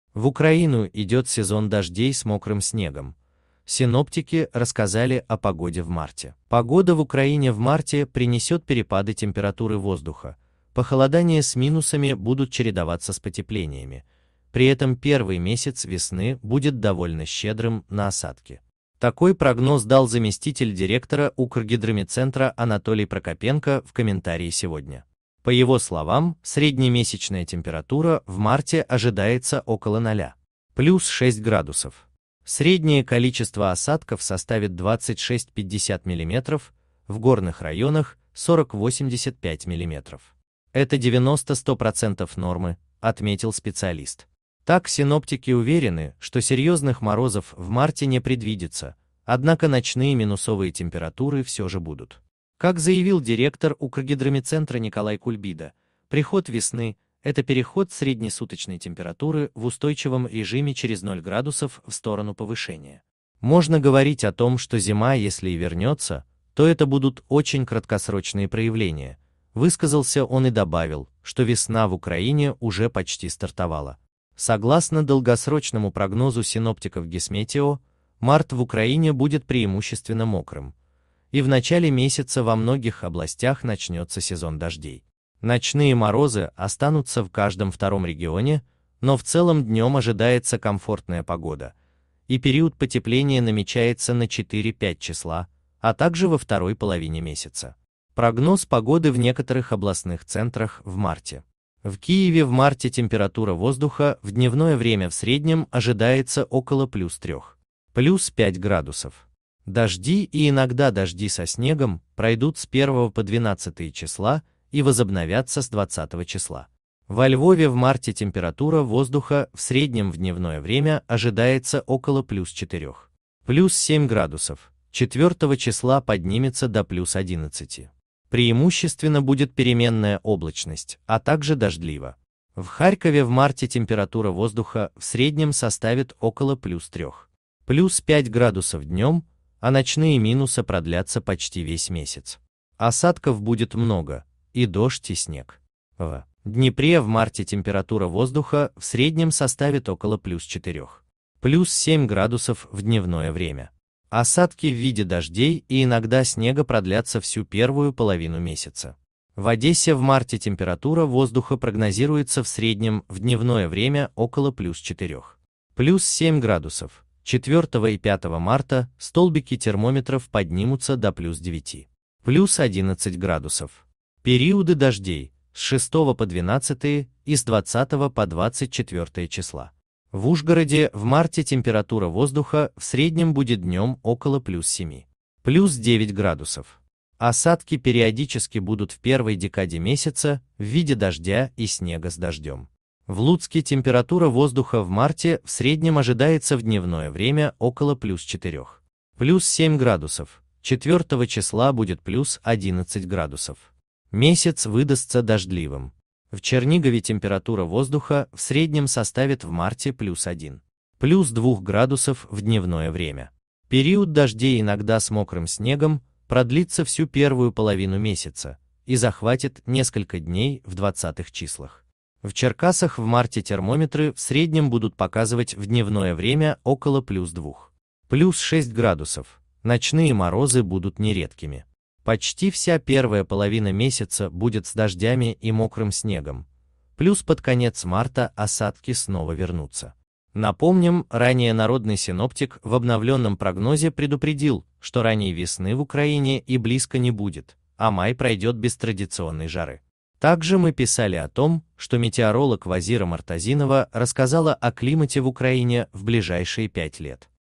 В Украину идет сезон дождей с мокрым снегом: синоптики рассказали о погоде в марте.
Новости Стильжизни Гидрометцентр синоптики Погода в Украине в марте принесет перепады температуры воздуха, похолодания с "минусами" будут чередоваться с потеплениями, при этом первый месяц весны будет довольно щедрым на осадки. Такой прогноз дал заместитель директора Укргидрометцентра Анатолий Прокопенко в комментарии "Сегодня". По его словам, среднемесячная температура в марте ожидается около 0. +6 градусов. "Среднее количество осадков составит 26-50 мм, в горных районах - 40-85 мм. Это 90-100% нормы", - отметил специалист. Так, синоптики у